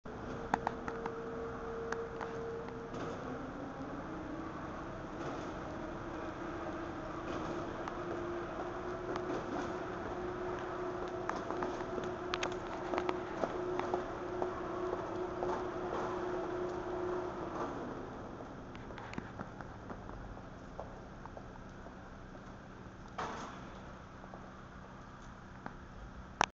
ソウル「高層アパートの引越し」
建物の外からはしごを使って引越しをしている。